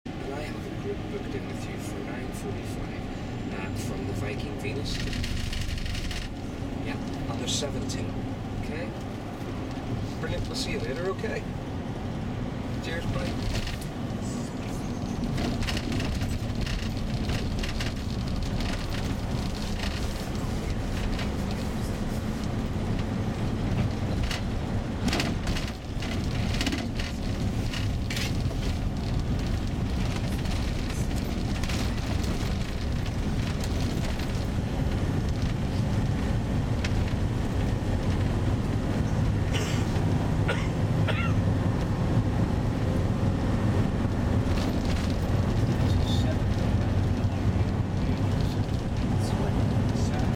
driver view bus ride through Balloo, N. Ireland, U.K. on the A22 on 5/20/23
This is a driver view bus ride through Balloo, N. Ireland, UK on the A22 on 5/20/23 during our excursion from Belfast down into County Down. You Will notice how the U.K does end of city limits signs differently than continental Europe with the 2 signs on both sides of the road at the very end which is different from the one sign with a line through it denoting the end of a town or village in most parts of mainland Europe.